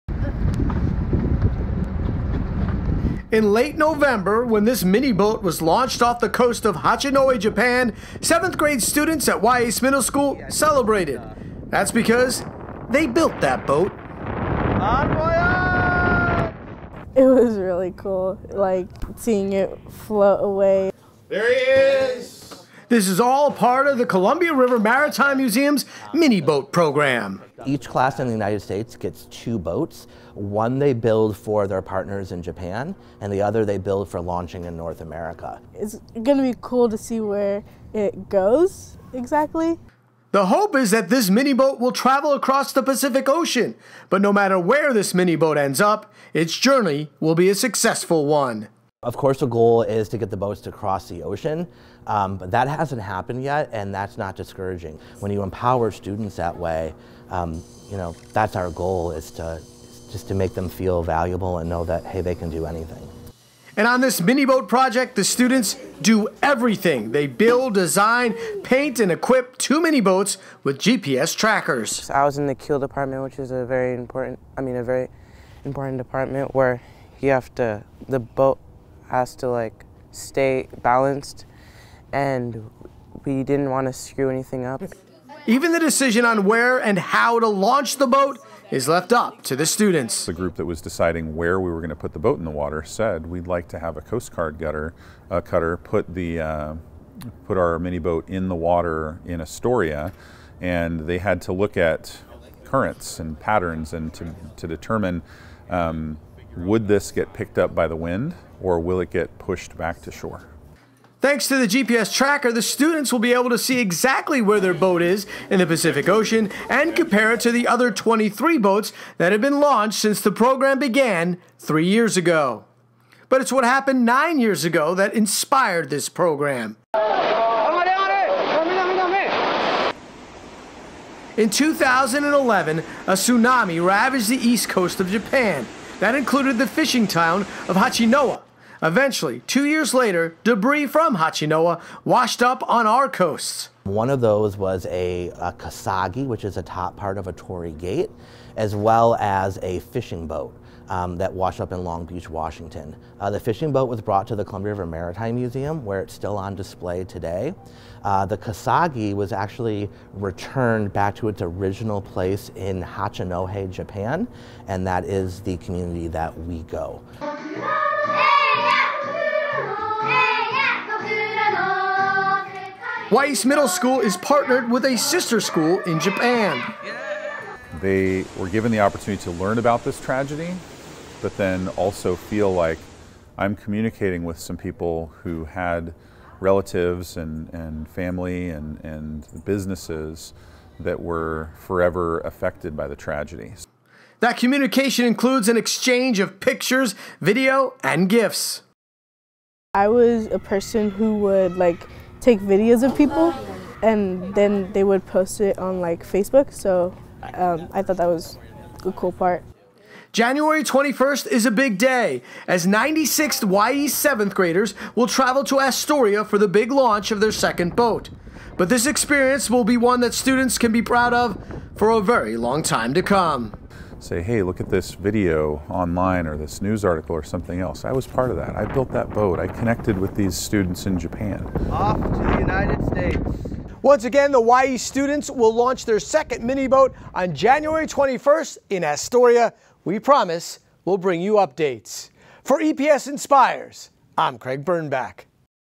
Mini-boats Spark Major Learning at Wy'east Middle School
7th graders at Wy'east Middle School are learning about natural disasters, engineering, ocean currents and Japanese culture. All this learning is a part of the Columbia River Maritime Museum's Mini-boat Program.